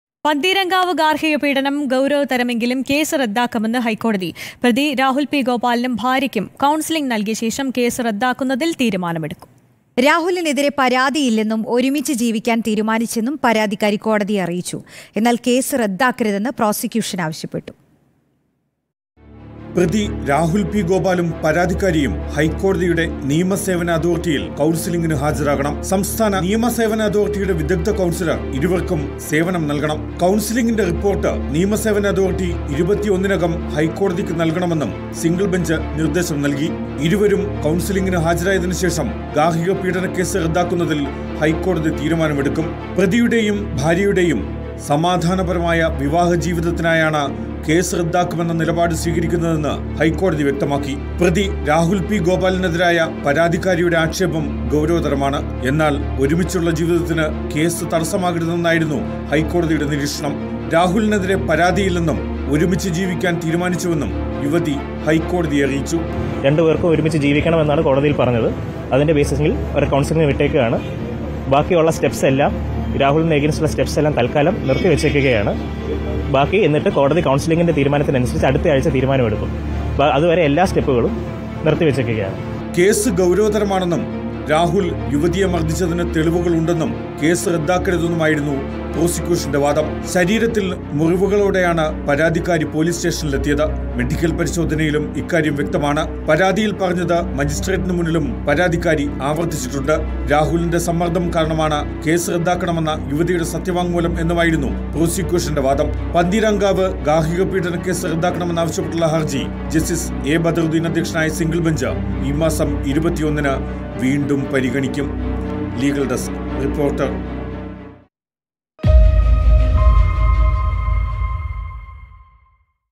പന്തീരാങ്കാവ് ഗാര്‍ഹിക പീഡനം ഗൗരവതരമെങ്കിലും കേസ് റദ്ദാക്കുമെന്ന് ഹൈക്കോടതി | High Court
പന്തീരാങ്കാവ് ഗാര്‍ഹിക പീഡനം ഗൗരവതരമെങ്കിലും കേസ് റദ്ദാക്കുമെന്ന് ഹൈക്കോടതി | High Court | Pantheeramkavu Case

With Regards
Team RBC